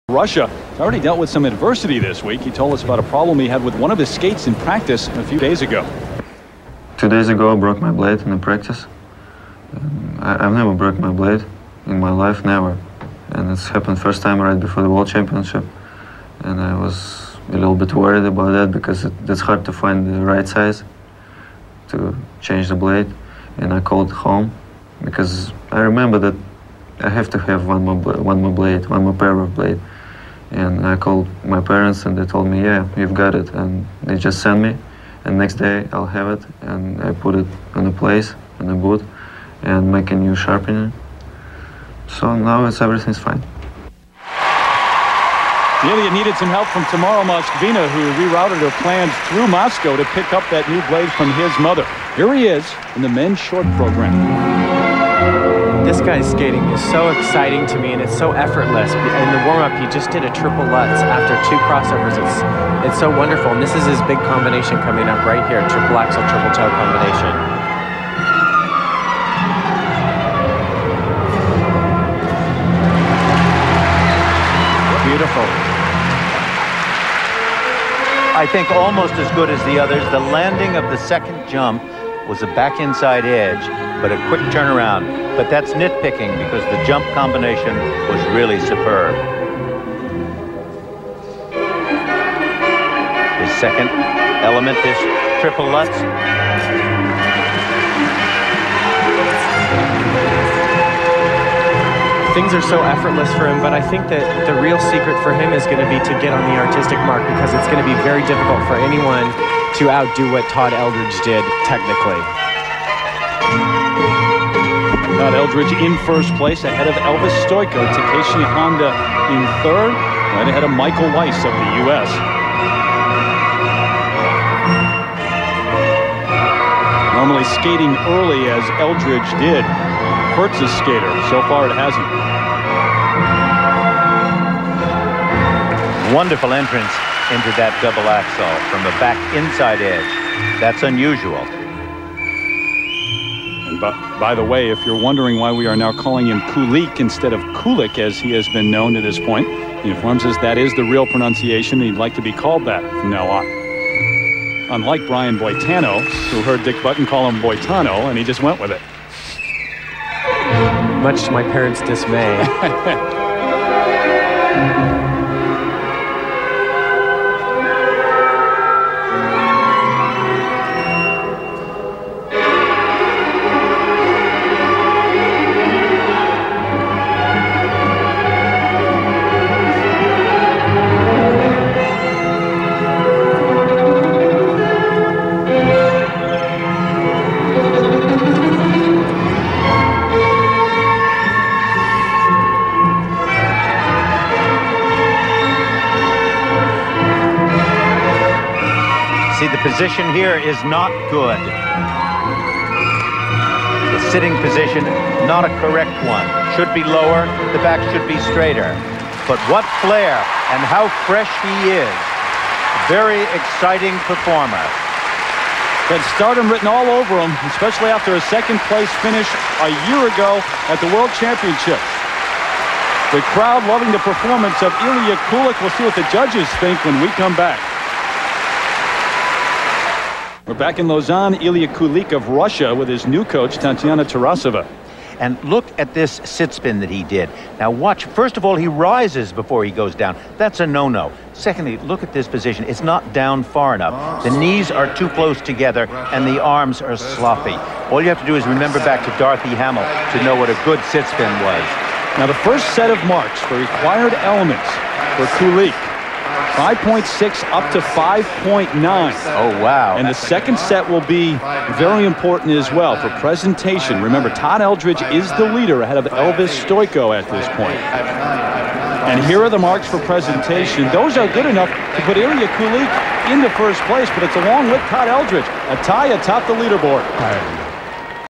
Ilia Kulik (RUS) - 1997 World Figure Skating Championships, Men's Short Program
Lausanne, SWITZERLAND - 1997 World Figure Skating Championships, Men's Short Program - Ilia Kulik of Russia placed 3rd after this skate.